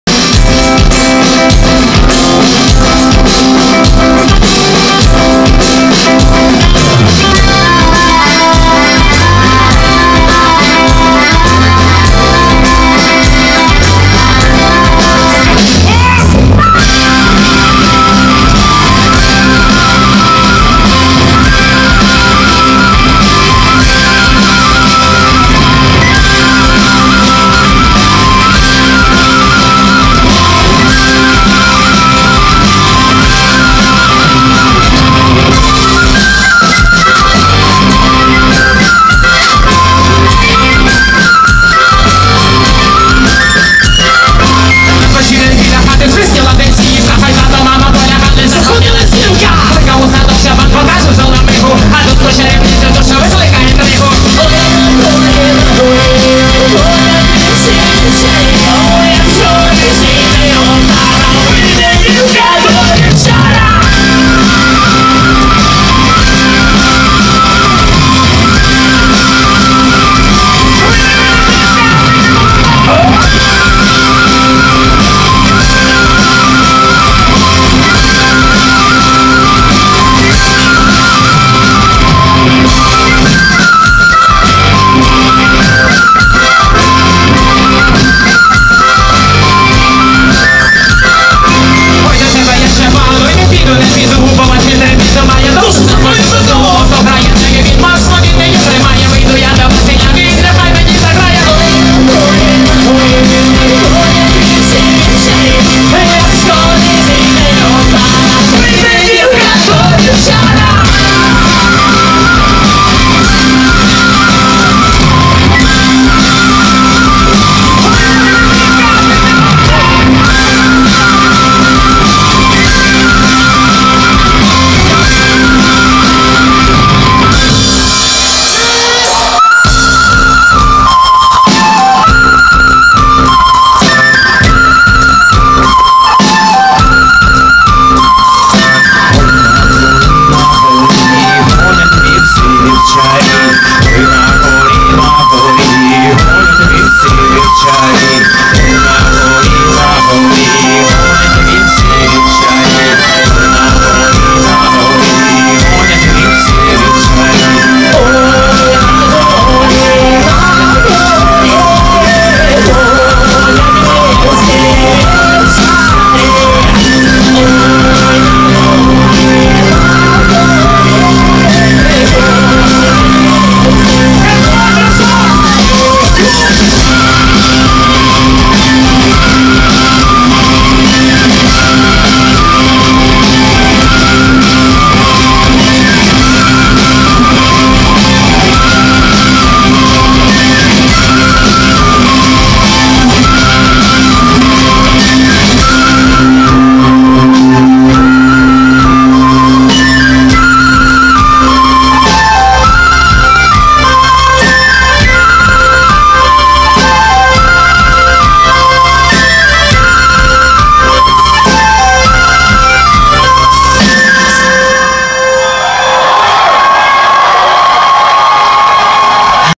кРок у майбутнє 2017 Виступ Ангажемент на вівторок
Виступ українського фольк-гурту "АНГАЖЕМЕНТ НА ВІВТОРОК" на ІХ Міжнародний рок-фестиваль "кРок у майбутнє" у Херсоні.
Автор відео Ілона Полесова